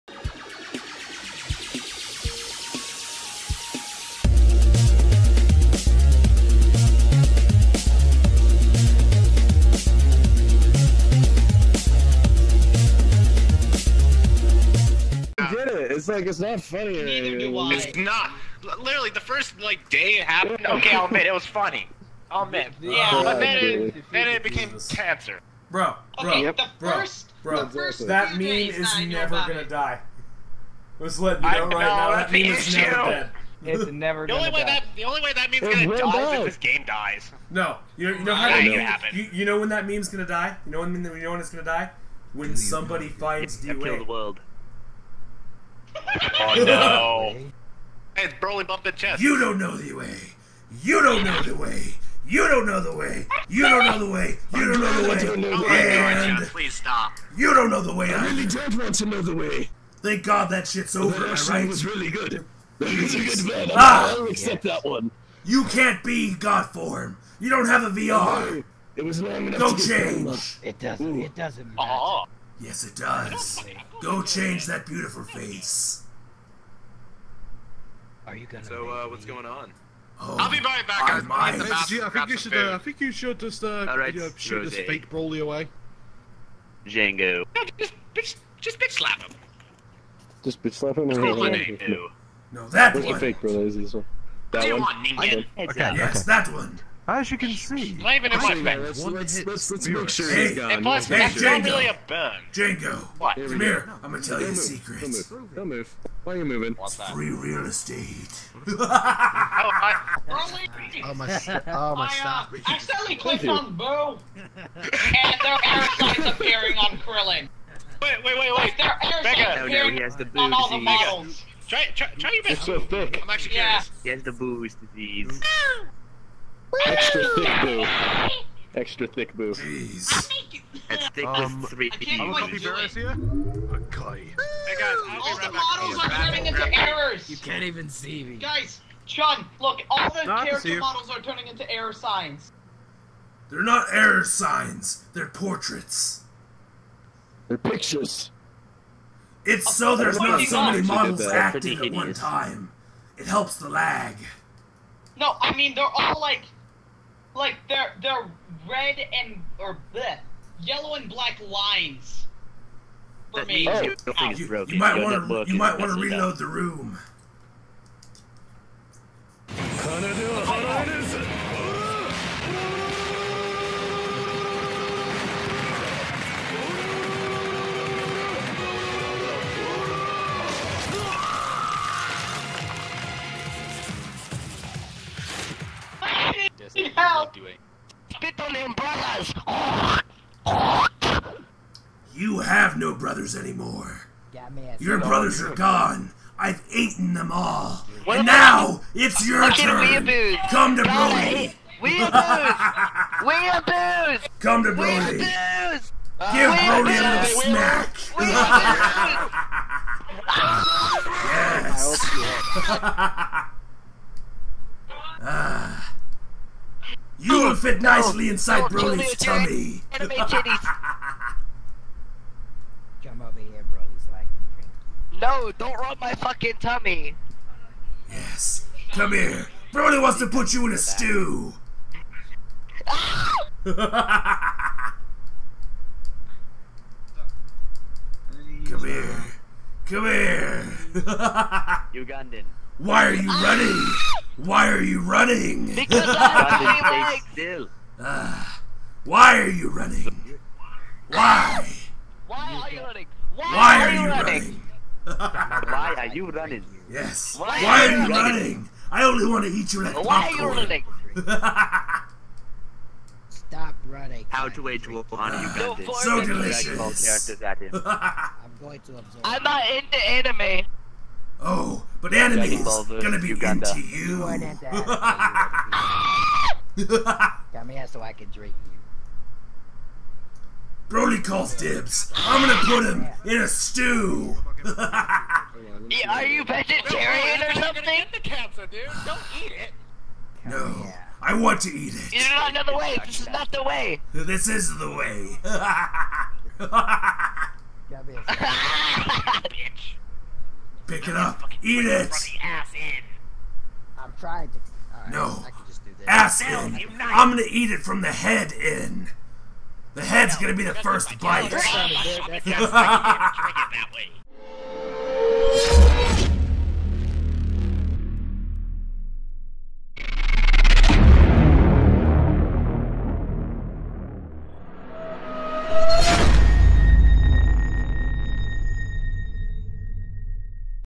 Broly Chases a Ugandan Knuckles! VRchat!
What fortune! Broly has found a lone Ugandan knuckles all by himself. LETS GET HIM!